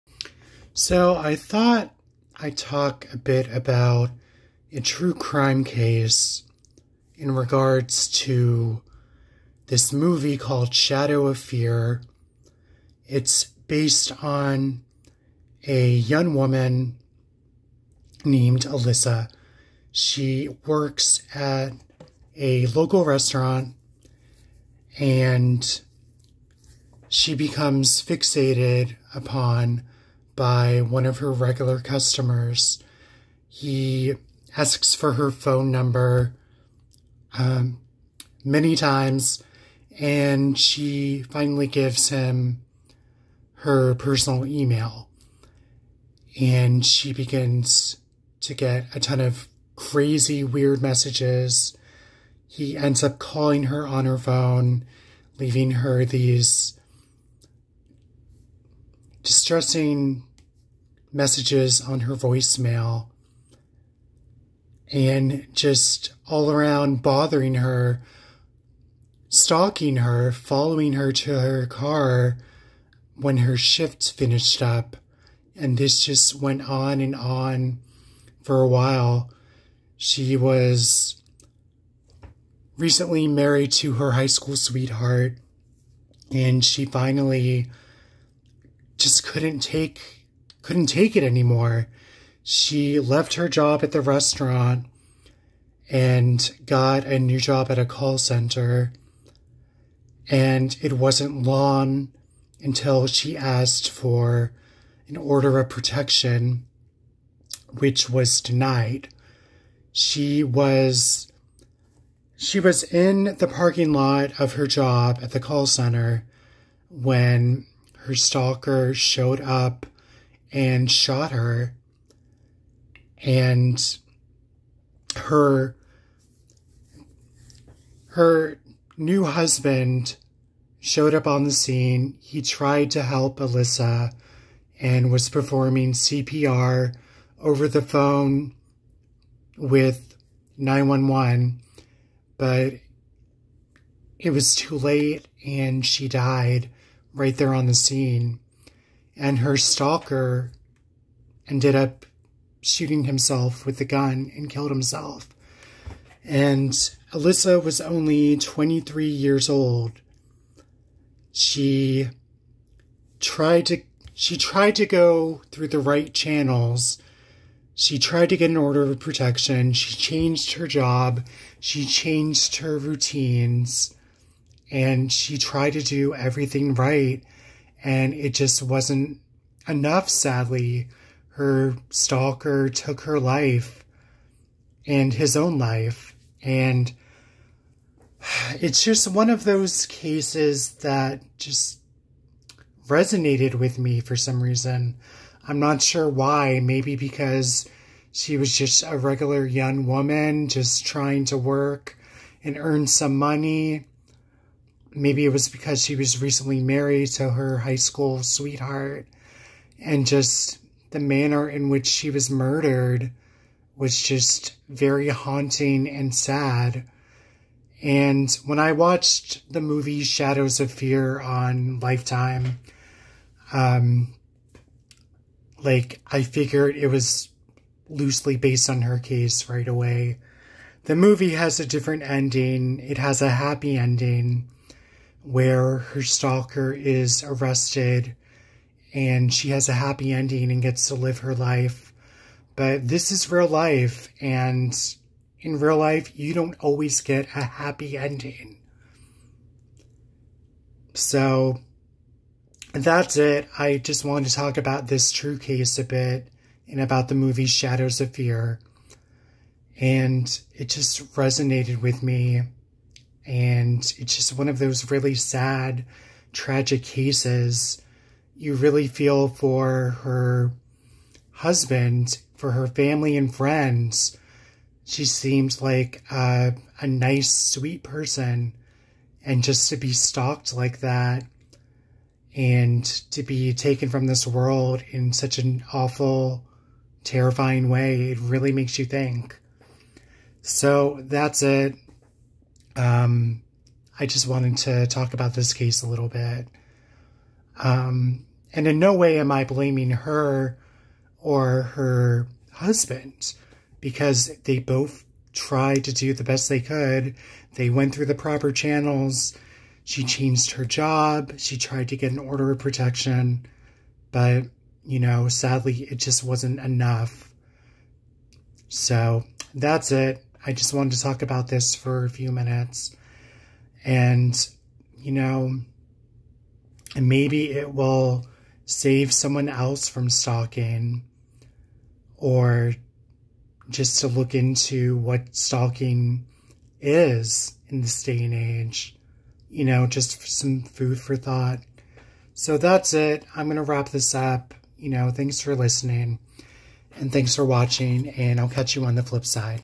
The Case that inspired "Shadow of Fear"
Talking about the True Crime Case that inspired this TV Movie.